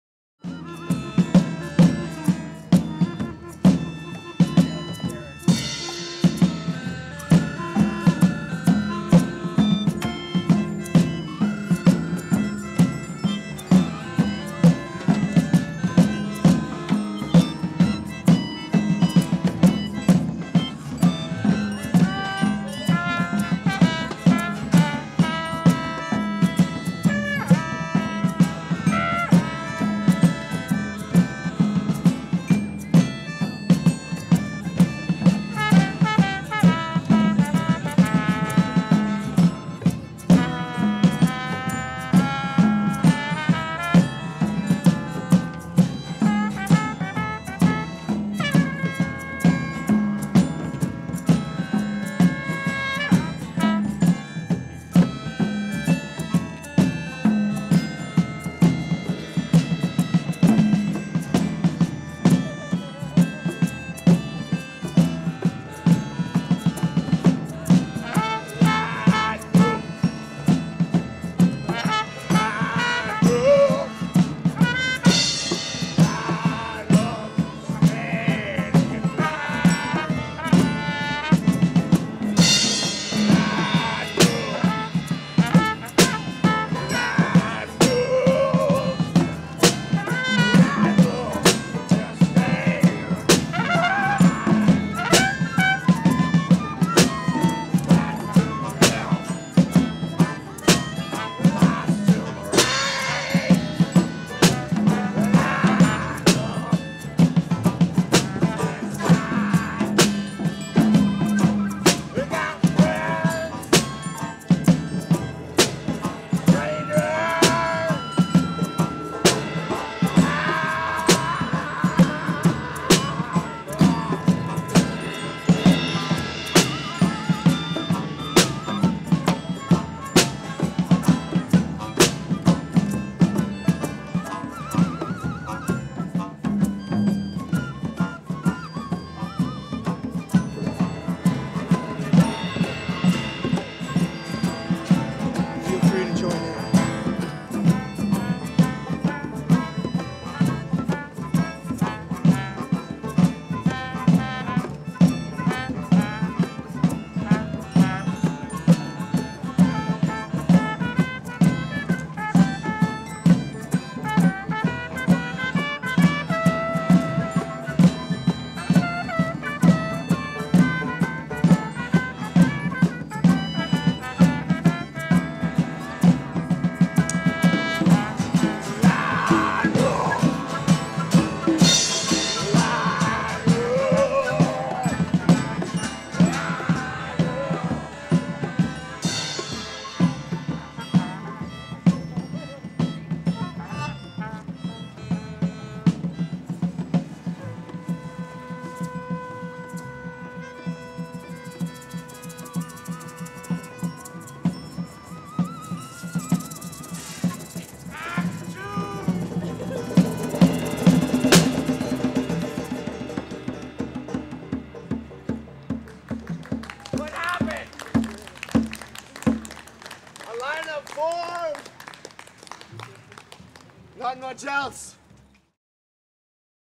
nuit blanche act 01
act one of the improvised puppet show/musical that basement arms did for nuit blanche 2007. it featured puppeteers from the troupe Animal Borderline. it got a little too dark to see anything.